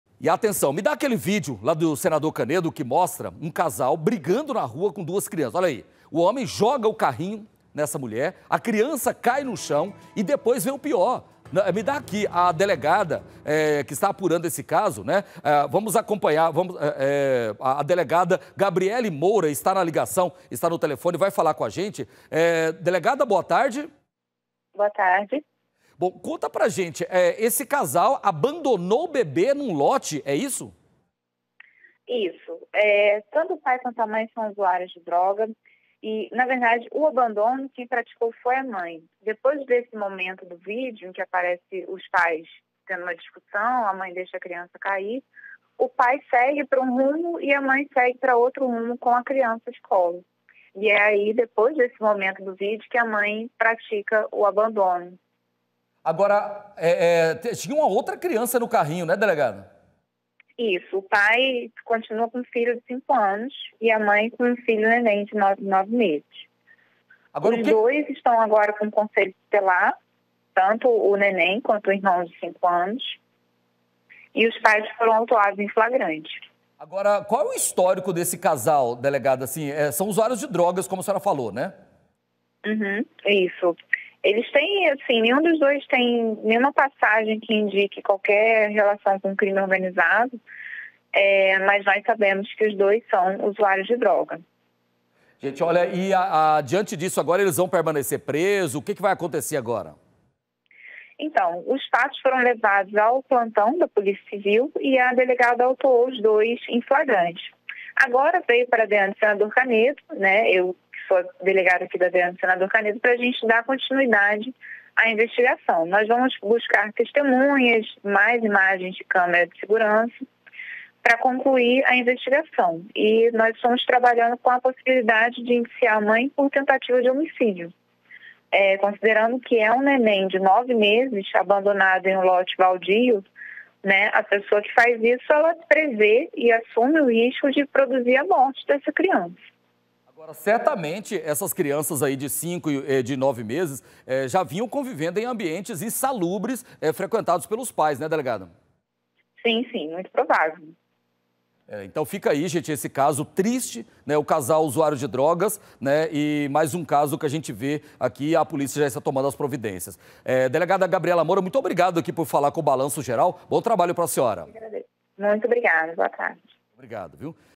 JUDIAÇÃO: BEBÊ ABANDONADO É RESGATADO EM LOTE
VOLTAMOS A FALAR DO BEBÊ ABANDONADO NUM LOTE EM SENADOR CANEDO. A DELEGADA GABRIELA MOURA QUE ESTÁ INVESTIGANDO O CASO.